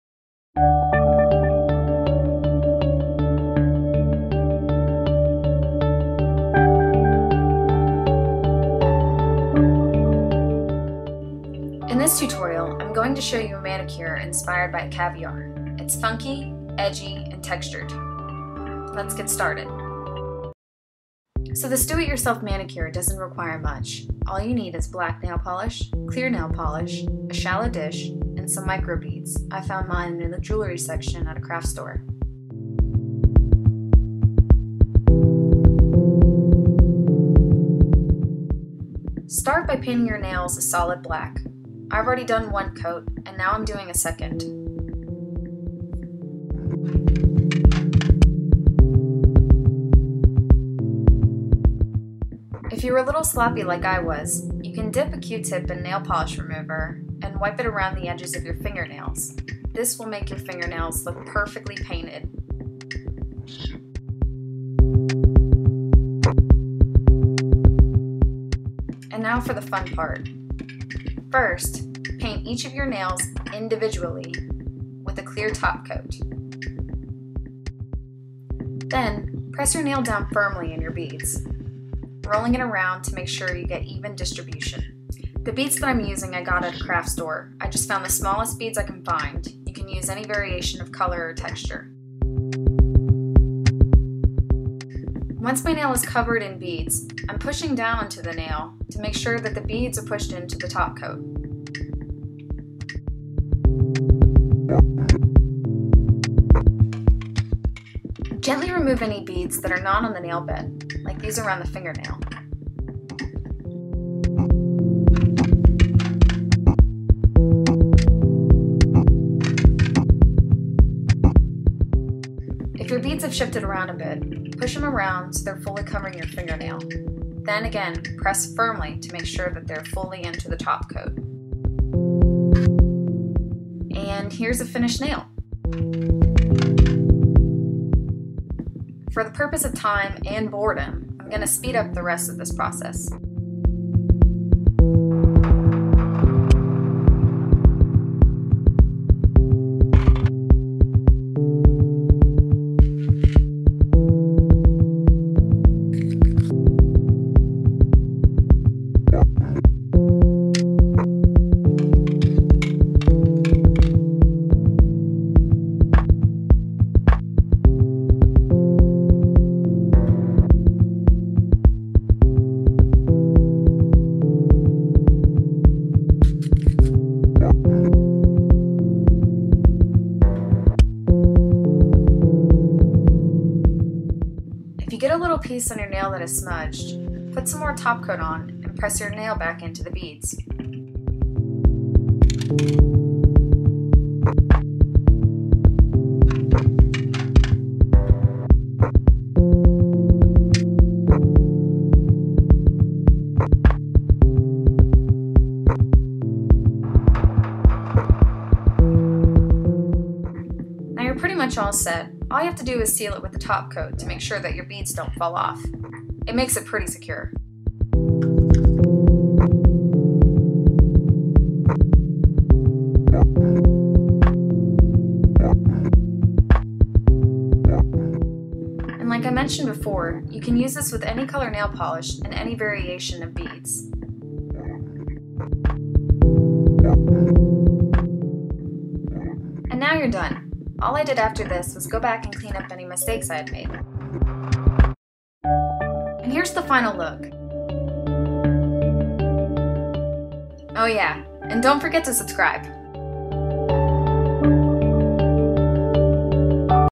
Caviar Inspired Nail Tutorial - 3D Nails
Textured, hip, funky nails inspired by the, ever-so-popular, beaded nails.
Supplies: nail polish (I used black, but any will do), Clear top coat, tiny tiny beads, small container to put beads in.
Check back soon for more nail, jewelry, make up, hair, and home tutorials! Anything to beutify it yourself!!!